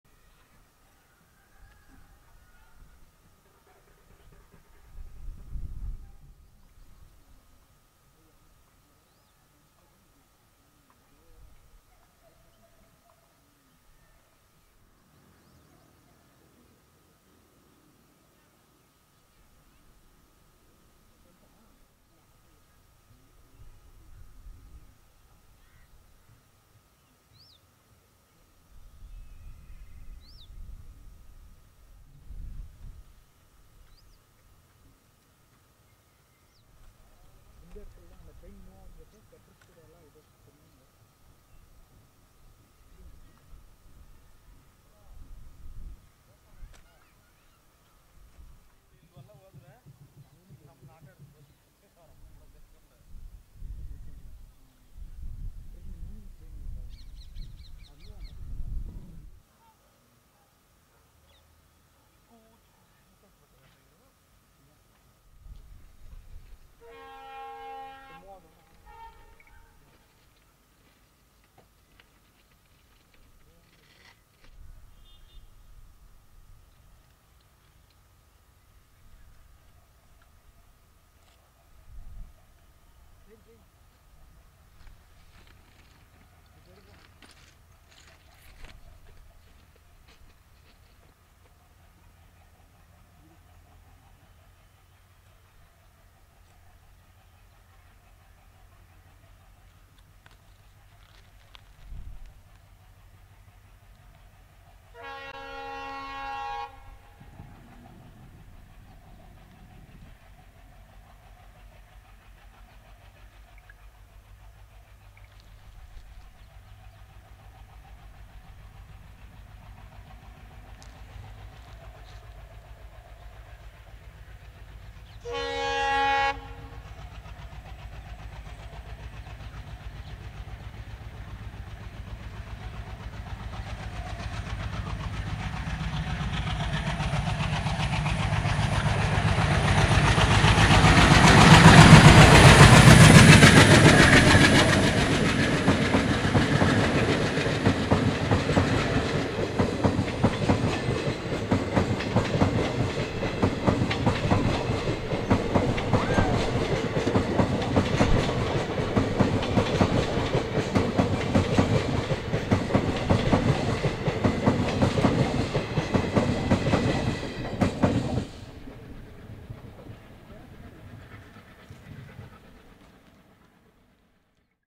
Classic KJM ALCO with Yeswantpur - Salem | Station Vibes
KJM wdm3a arrival & departure at hosur with Yeswantpur - Salem express